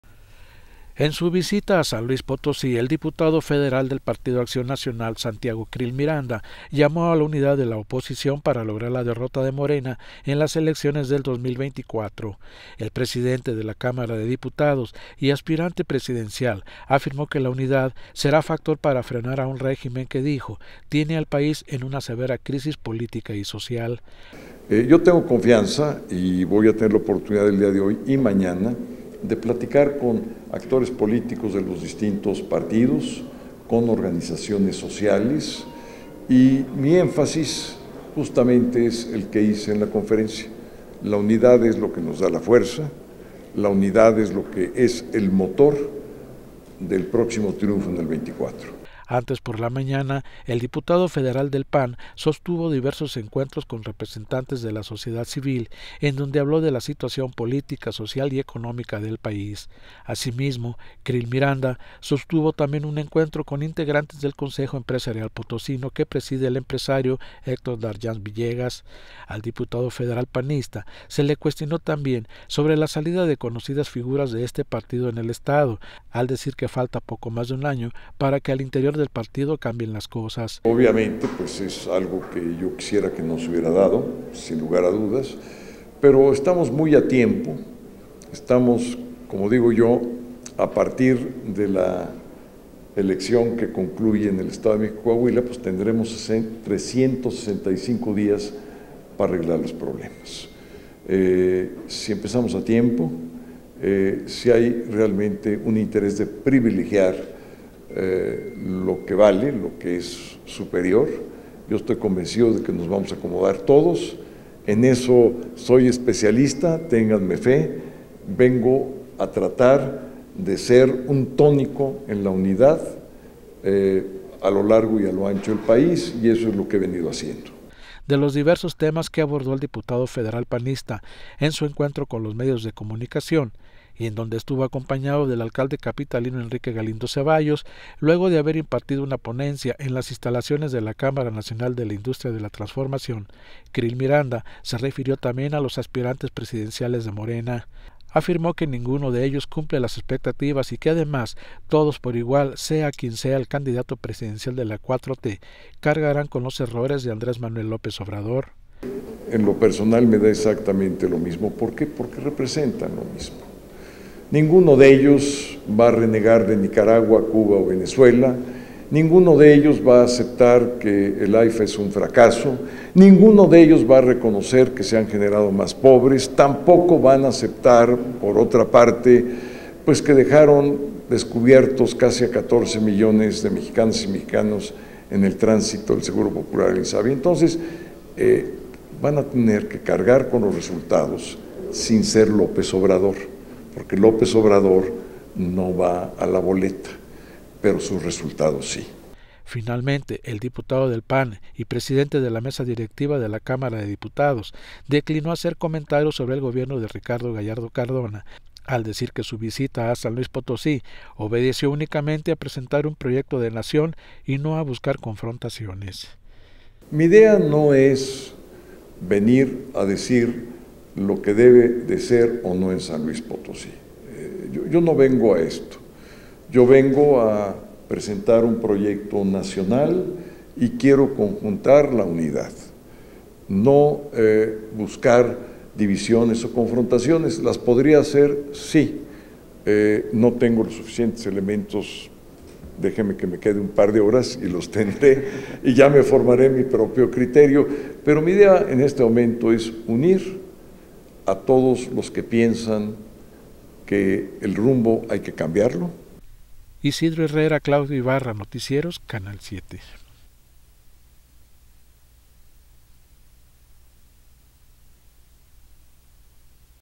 Pide Santiago Creel en SLP unidad en la oposición para derrotar a MORENA
SLP.- En su visita a San Luis Potosí el diputado federal del Partido Acción Nacional Santiago Creel Miranda llamó a la unidad de la oposición para lograr la derrota de MORENA, en las elecciones del 2024.

El presidente de la Cámara de Diputados y aspirante presidencial afirmó que la unidad será factor para frenar a un régimen que dijo, tiene al país en una severa crisis social y política.

Antes, por la mañana, el diputado federal del PAN sostuvo diversos encuentros con representantes de la sociedad civil en donde habló de la situación política, social y económica del país.
Asimismo, Creel Miranda sostuvo también un encuentro con integrantes del Consejo Empresarial Potosino que preside el empresario Héctor D’Argance Villegas.
Al diputado federal panista se le cuestionó también sobre la salida de conocidas figuras de este partido en el estado al decir que falta poco más de un año, para que al interior del estado cambien las cosas.

De los diversos temas que abordó el diputado federal panista en su encuentro con los medios de comunicación y en donde estuvo acompañado del alcalde capitalino Enrique Galindo Ceballos, luego de haber impartido una ponencia en las instalaciones de la Cámara Nacional de la Industria de la Transformación, Creel Miranda se refirió también a los aspirantes presidenciales de MORENA.

Afirmó que ninguno de ellos cumple las expectativas y que además, todos por igual sea quien sea el candidato presidencial de la 4T, cargarán con los errores de Andrés Manuel López Obrador.

Finalmente, el diputado del PAN y presidente de la Mesa Directiva de la Cámara de Diputados declinó hacer comentarios sobre el gobierno de Ricardo Gallardo Cardona al decir que su visita a San Luis Potosí, obedeció únicamente a presentar un proyecto de nación y no a buscar confrontaciones.